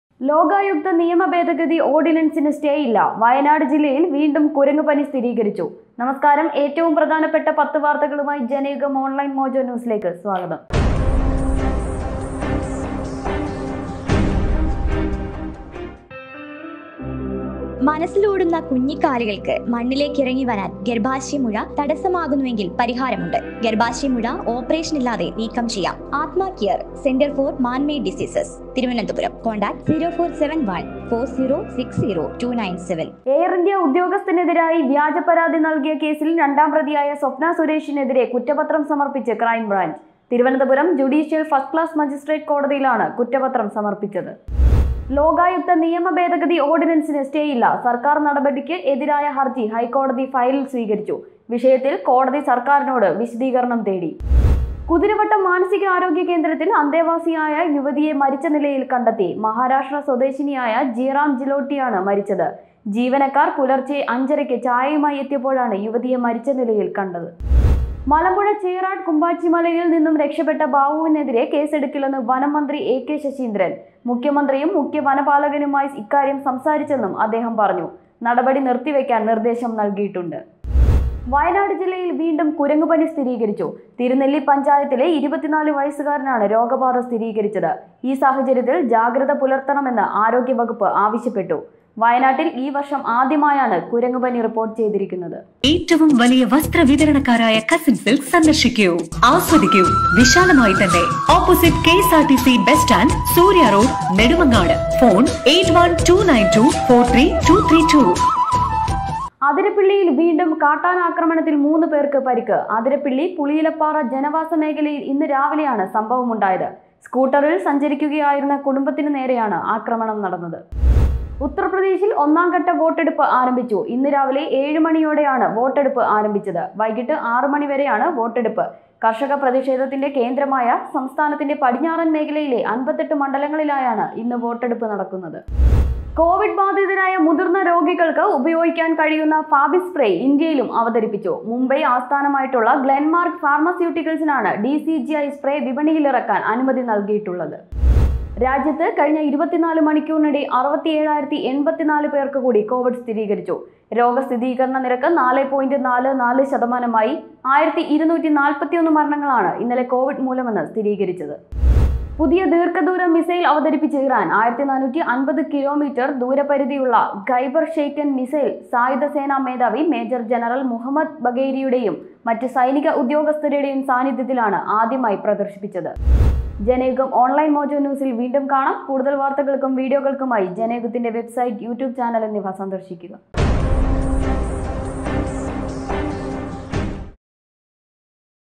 വയനാട് ജില്ലയില്‍ വീണ്ടും കുരങ്ങുപനി | MOJO News
വയനാട് ജില്ലയില്‍ വീണ്ടും കുരങ്ങുപനി

ലോകായുക്ത നിയമഭേദഗതി ഓര്‍ഡിനന്‍സിന് സ്‌റ്റേയില്ല